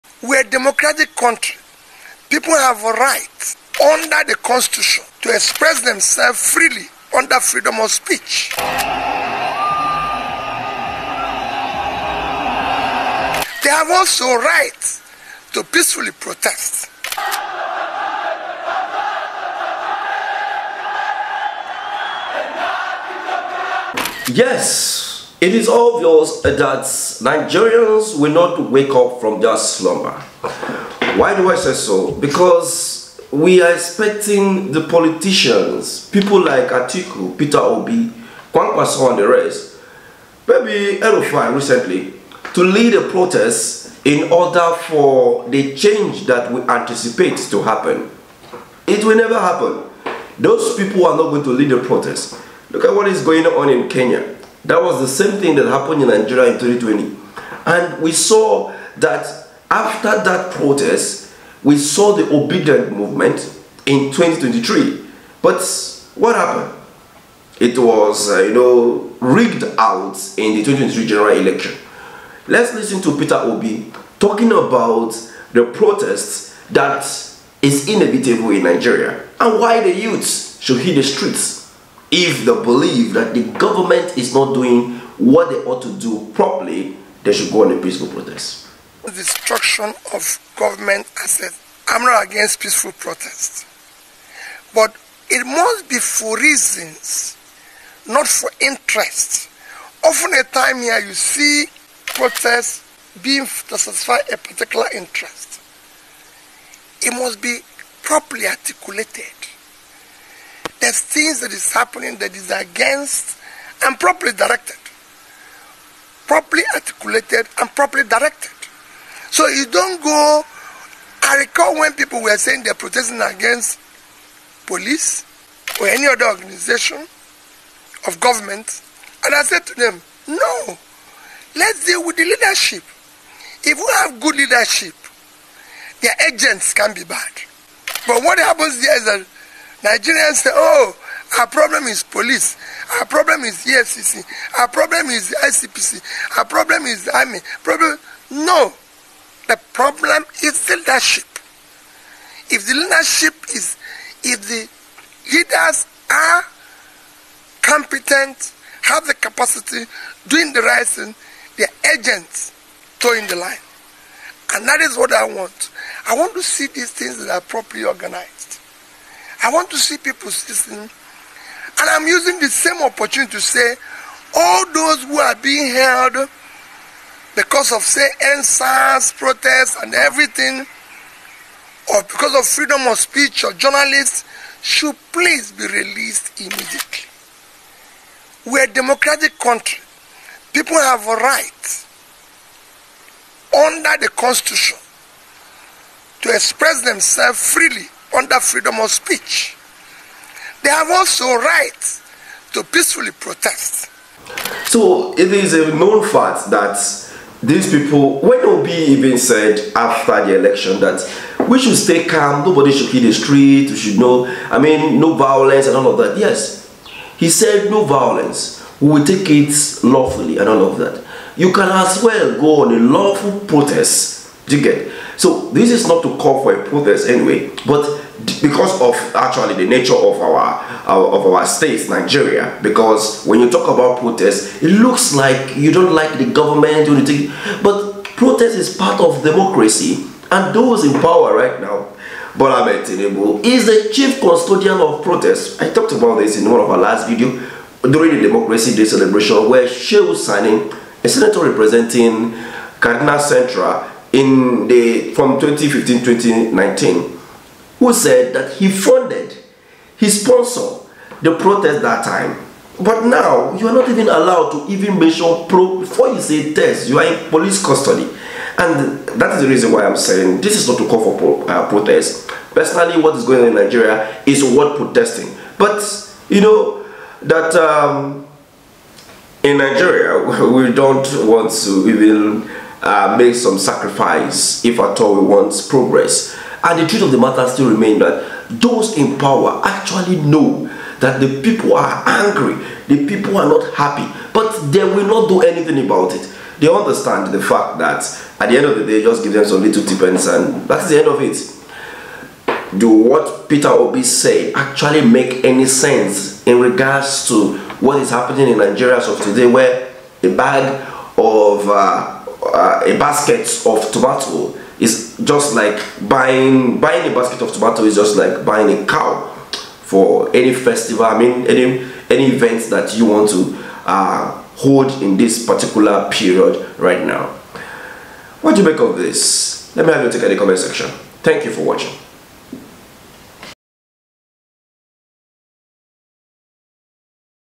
Peter Obi Set To Led A Peaceful Protest Against BAD Government.
Peter Obi Set To Led A Peaceful Protest Against BAD Government. according to him its a right to protest peacefully and that we are in a democratic government.